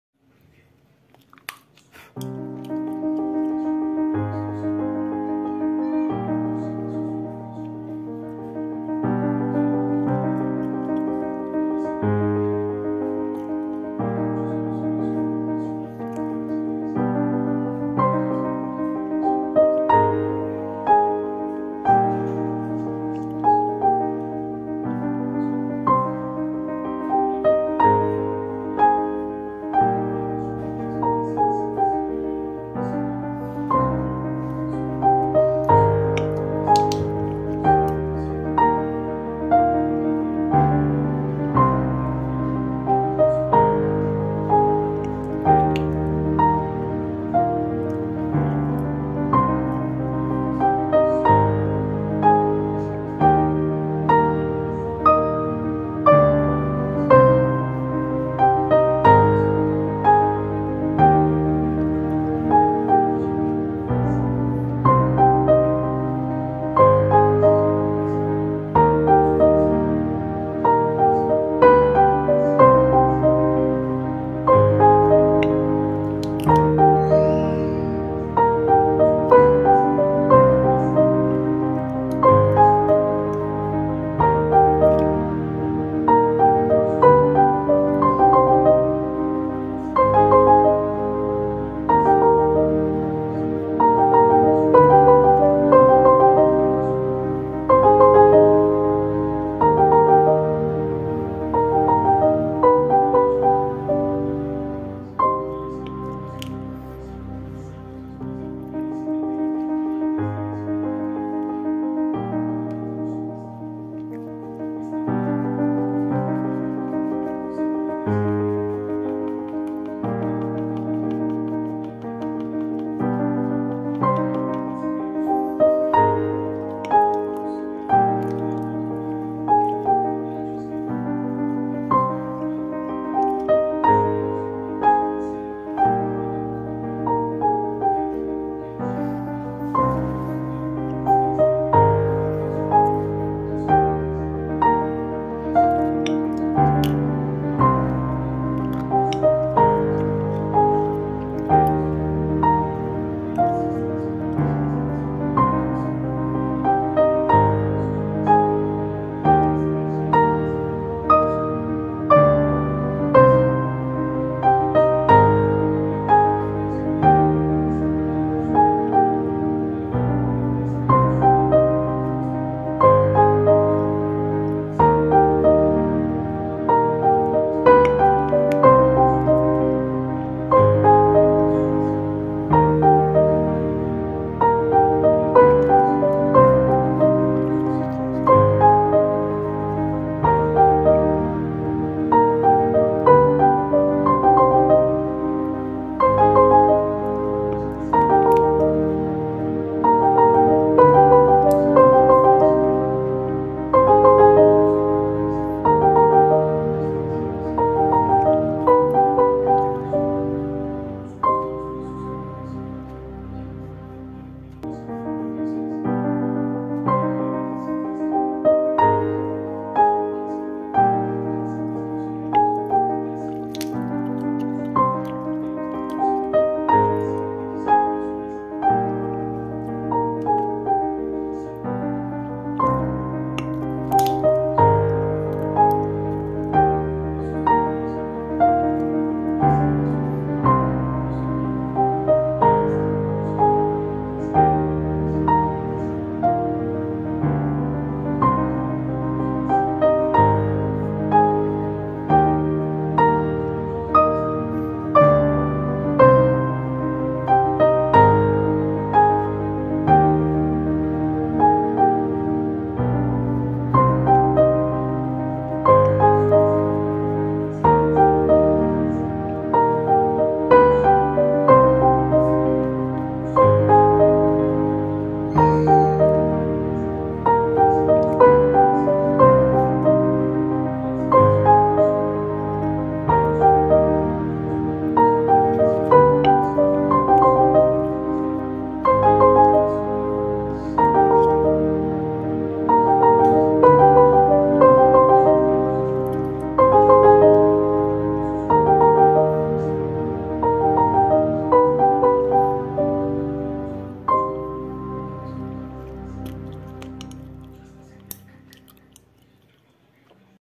como desenhar um coelho de páscoa?
Na religião cristã, os ovos também representam a ressurreição de Cristo, que é celebrada na Páscoa. À medida que o cacau se tornou mais acessível, surgiu a ideia de escavar ovos para recheá-los com chocolate. Felizmente para o nosso paladar!

-Materiais:
-Pencil 2B
Folha -A4